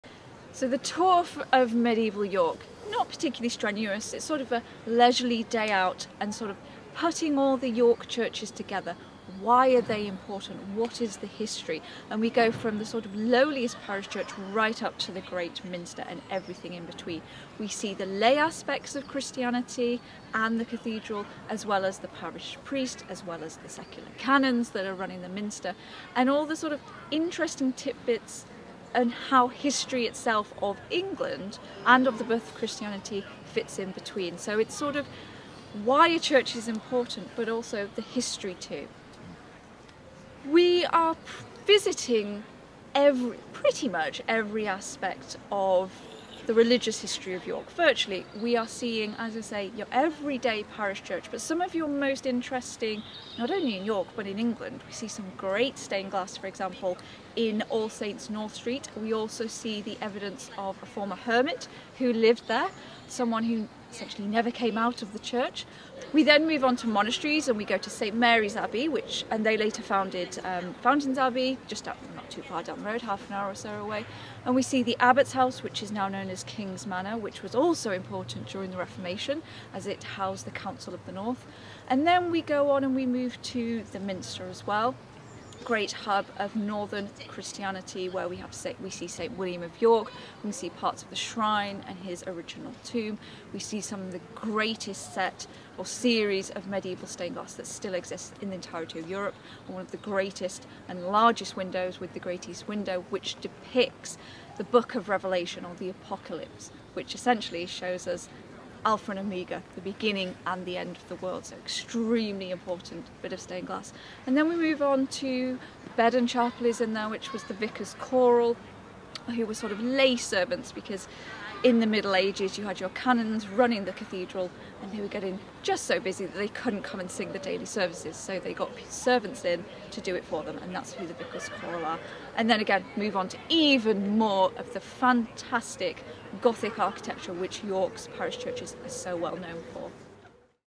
The Churches of Medieval York: tour Introduction from Dr Emma Wells
Dr Emma Wells introduces her four-day walking tour of the Churches of Middle Ages York, the medieval centre of Christianity in the North. She describes how this small-group, expert-led tour visits the city's architectural gems, and describes how to “read” a church and discover countless stories of the daily lives, experiences and beliefs of people past, from prince to pauper.